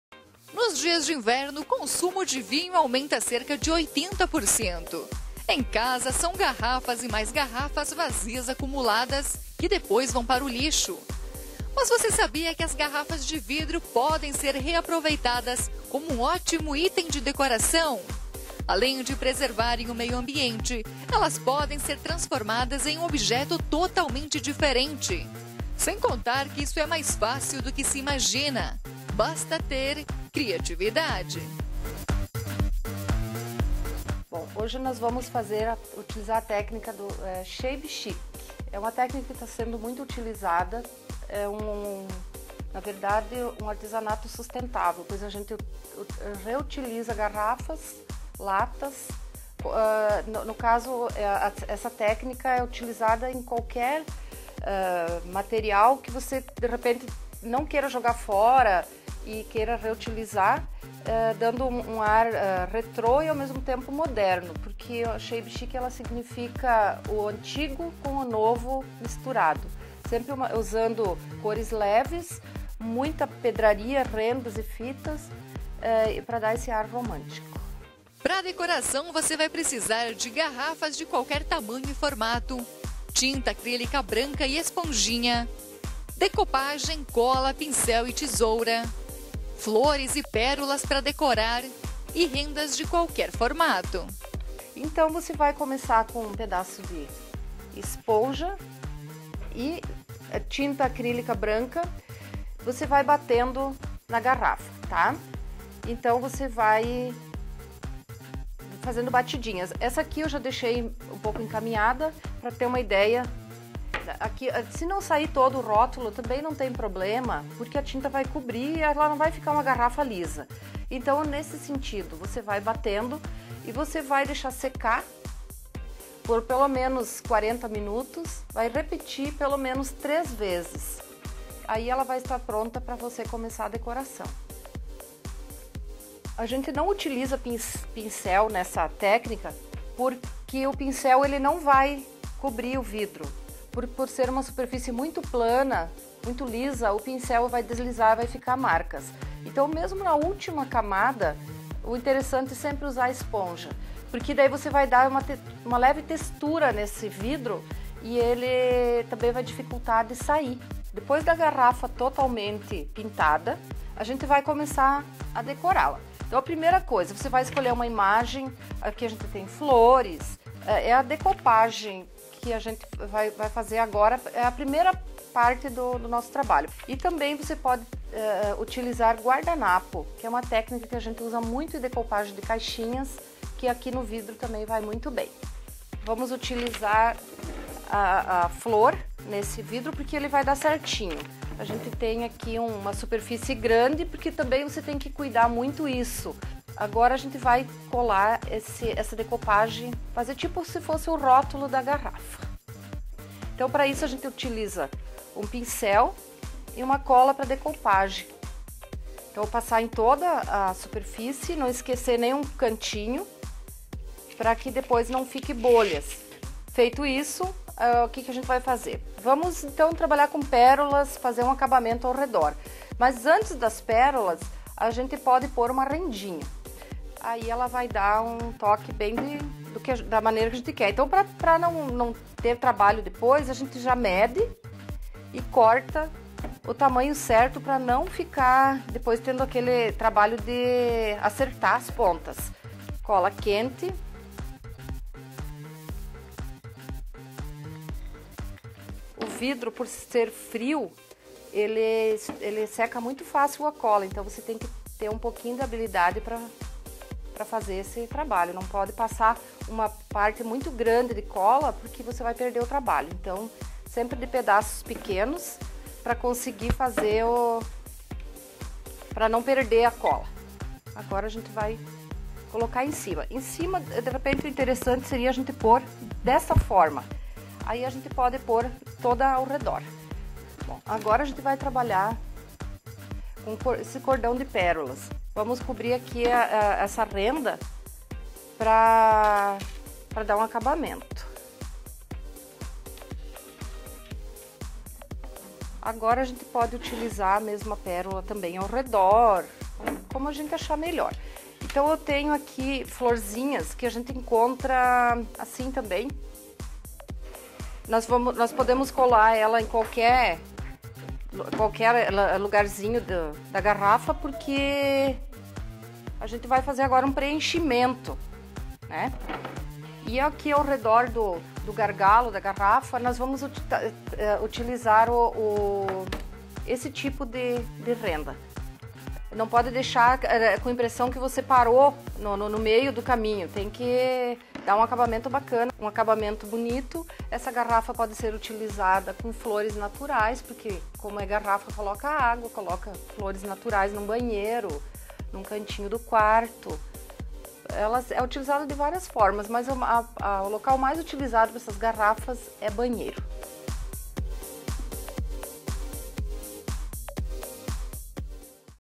Artesanato: Saiba como reutilizar garrafas de vidro
Uma alternativa ambientalmente correta e barata para decorar sua casa, é o artesanato em garrafas de vidro. Aprenda como fazer!